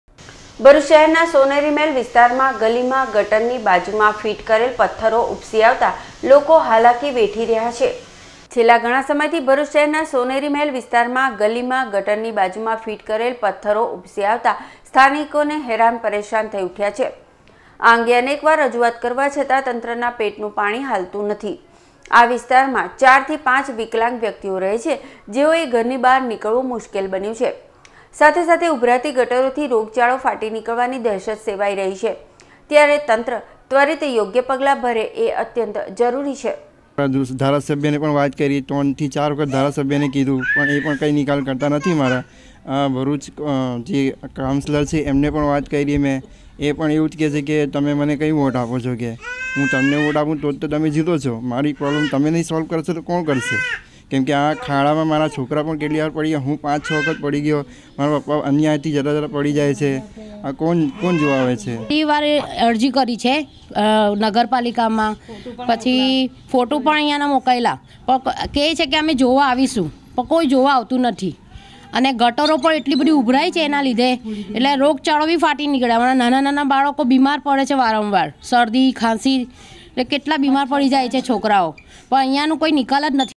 Narmada Samachar
ભરૂચ શહેરના સોનેરી મહેલ વિસ્તારમાં ગલીમાં ગટરની બાજુમાં ફીટ કરેલ પથ્થરો ઉપસી આવતા લોકો હાલાકી